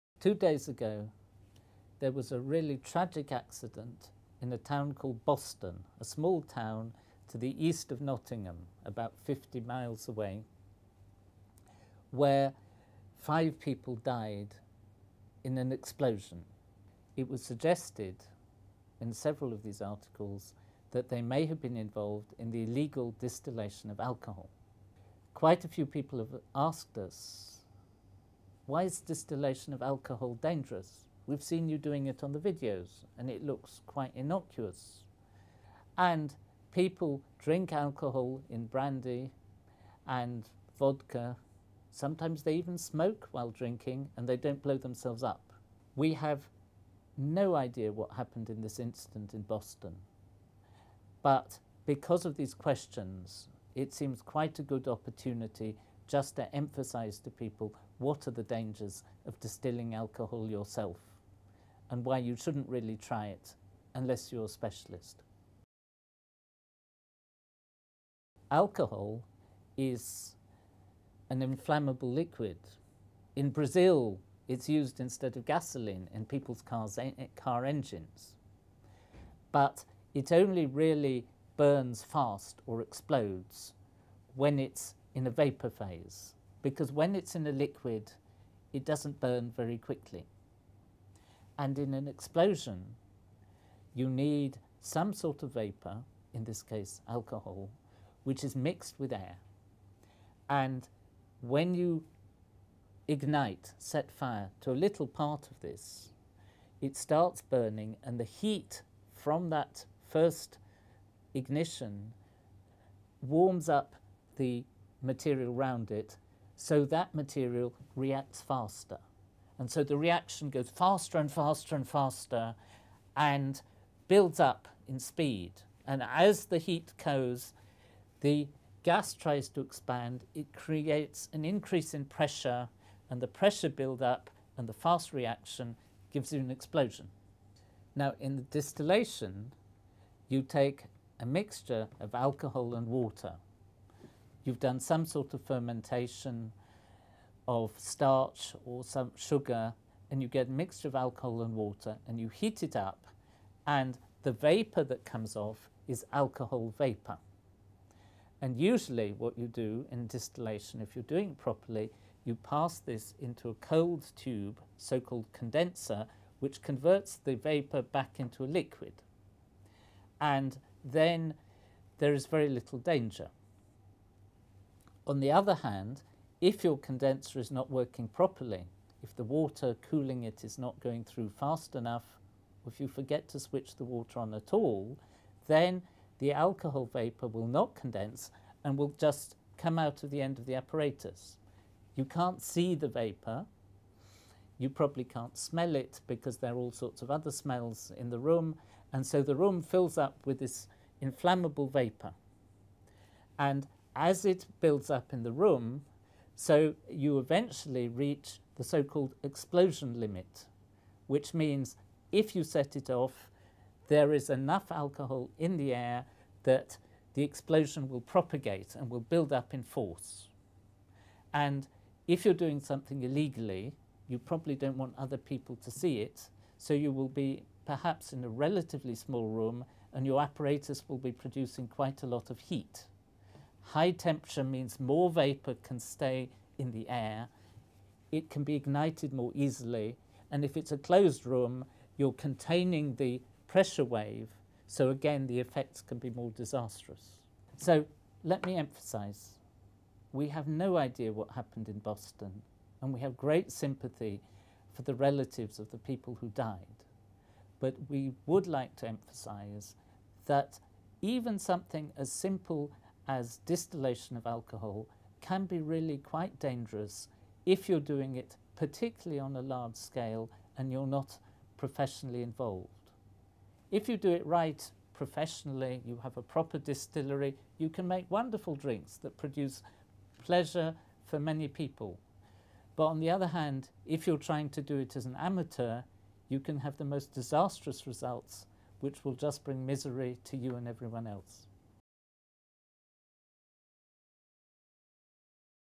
Distillation Danger - Periodic Table of Videos
In the wake of a tragic explosion in Boston, Lincolnshire, we discuss some of the dangers involved with distilling alcohol. We must stress we do not know the circumstances of this specific incident. However all the media coverage makes it seem an appropriate time to briefly discuss some of the issues involved.